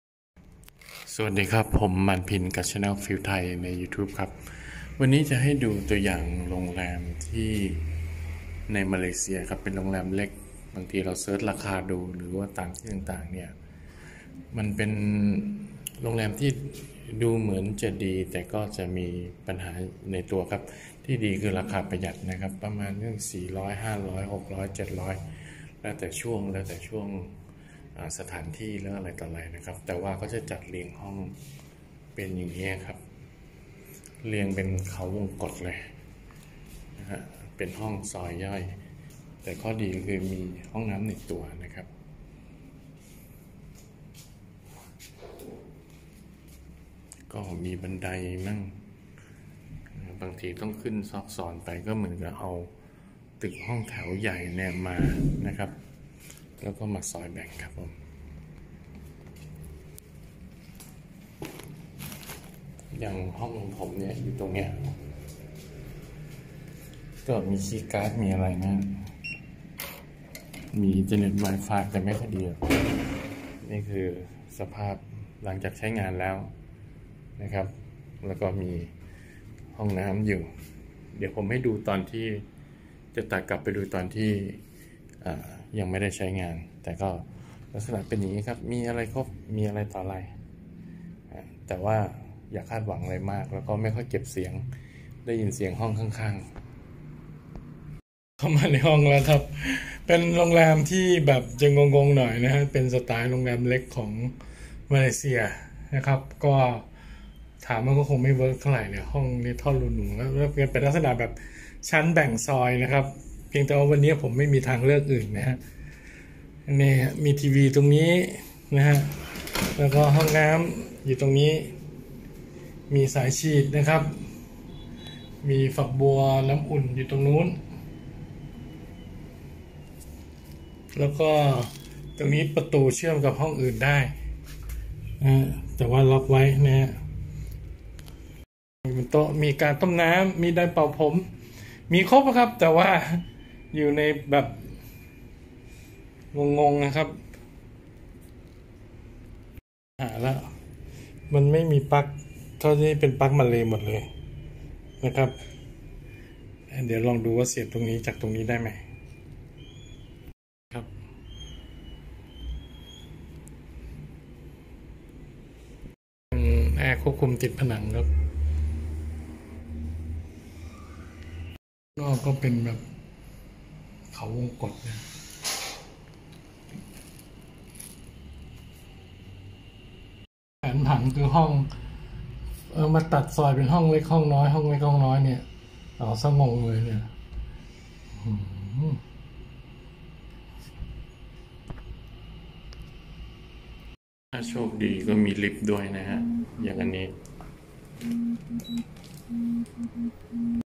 โรงแรมตึกแถวแบ่งห้อง เจอบ่อยในมาเลเซีย
พามารู้จักลักษณะโรงแรมที่เป็นตึกแถวแบ่งห้อง ราคาจะถูกกว่า แต่บางทีก็ไม่ดีเสมอไป จะเป็นห้องของตึกแถวมาซอยแบ่งกันครับ